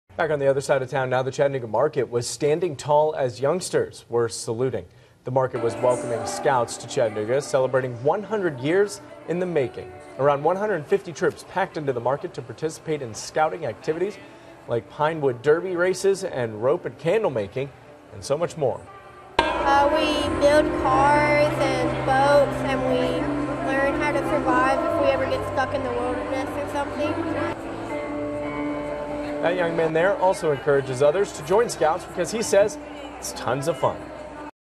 Celebrating 100 years!
The Chattanooga market was welcoming Boy Scouts to Chattanooga, celebrating 100 years in the making!

Around 150 troops packed into the market to participate in scouting activities, like pine wood derby races and rope and candle making and so much more.

"We build cars and boats and we learn how to survive if we ever get stuck in the wilderness," said Logan Spitznagel a cub scout in pack 3007.

Logan also encourages others to join scouts because he says it's tons of fun.